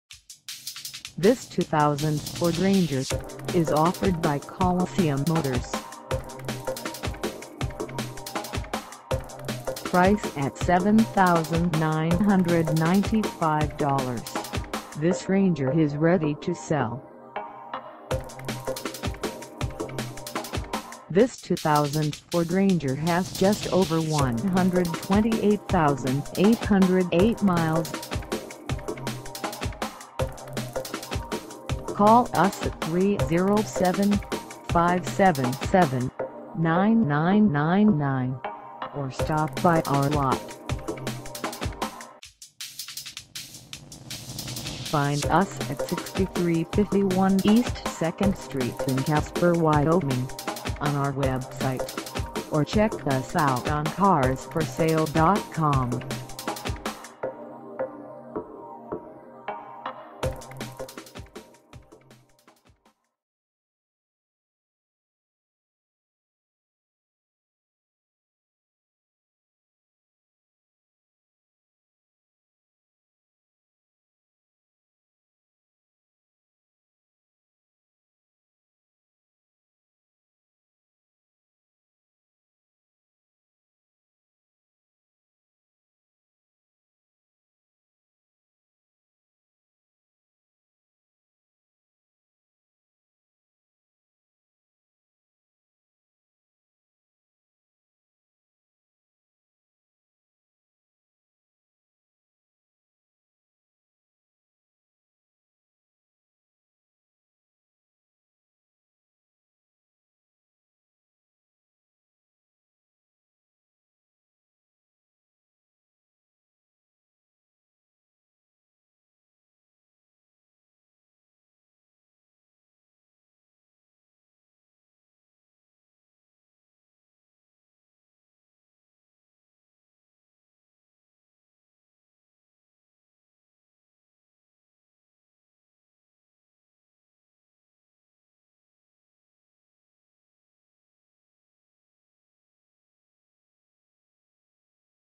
2000 Ford Ranger XL - for sale in Casper, WY 82609
Coliseum Motors
6351 E 2nd St

Come test dirve this 2000 Ford Ranger XL for sale in Casper, WY.